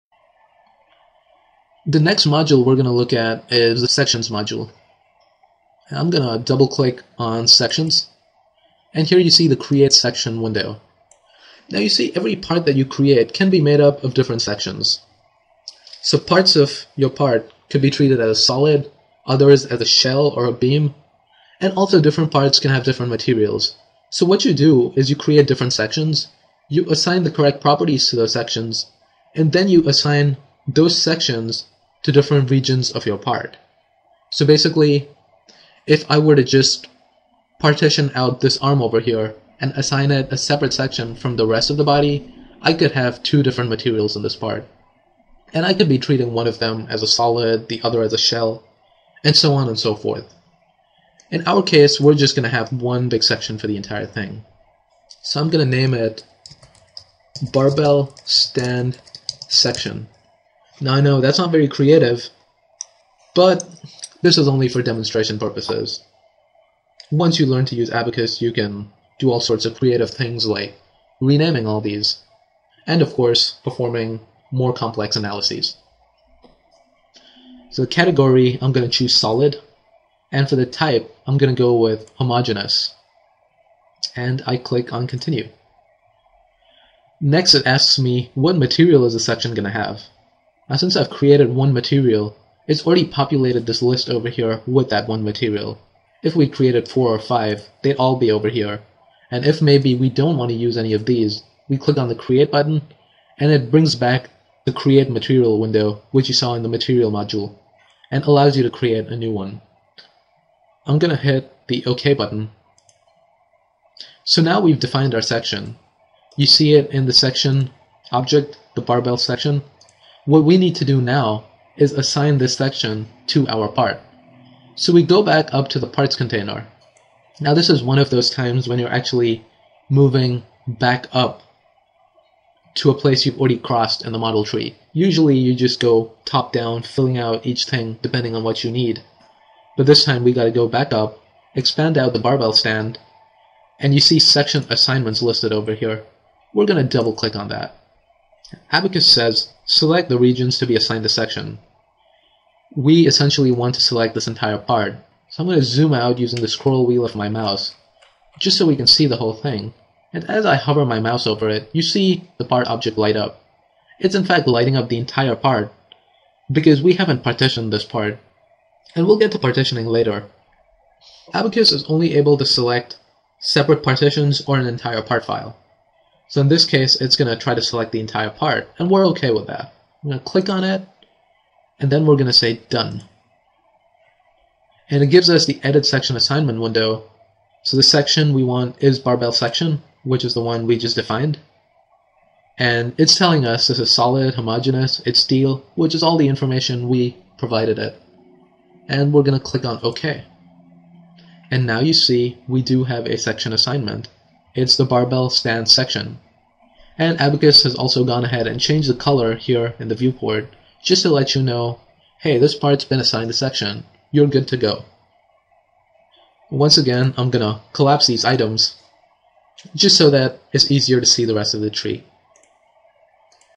1.h) Abaqus Basics - Create and assign a Section
This is a free tutorial on the basics of running a simulation in Abaqus. More information about this simulation is available